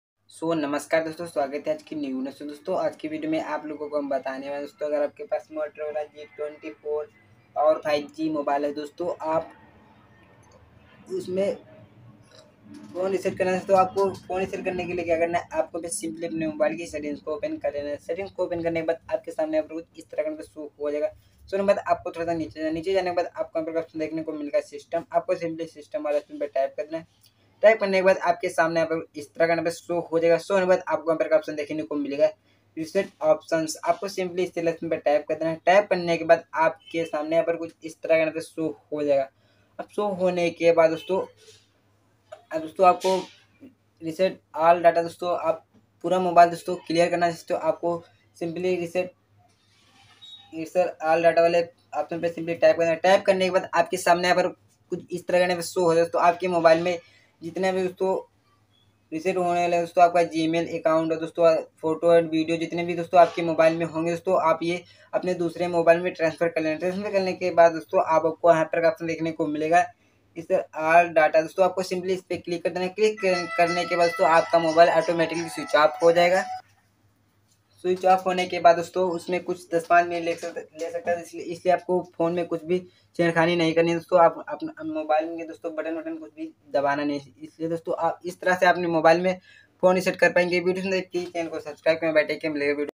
Moto g24 power me hard reset kaise kare/how to fector reset in moto g24 power me / reset kaise kare
Moto g24 power me hanging problem kaise shi kare